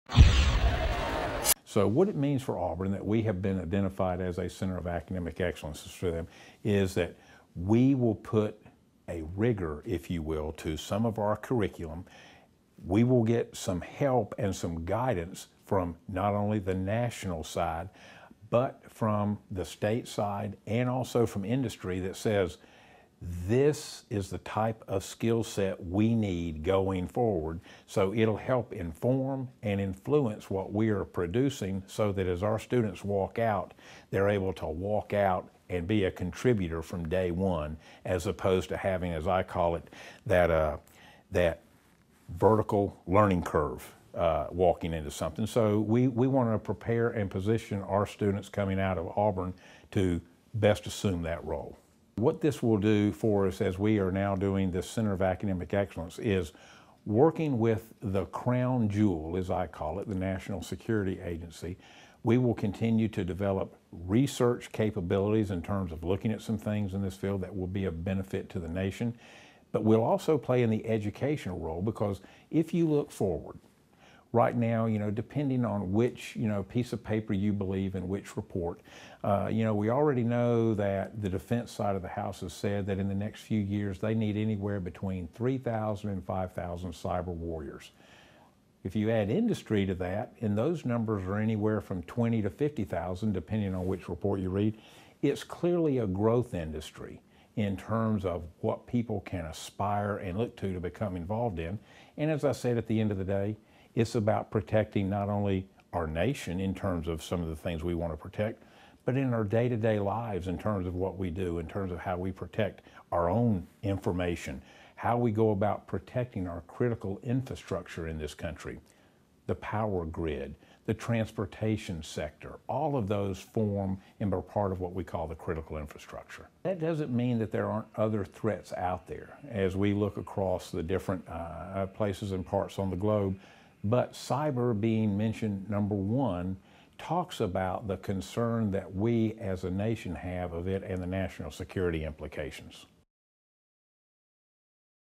National Recognition
The National Security Agency designates Auburn as a National Center of Academic Excellence in Cyber Operations.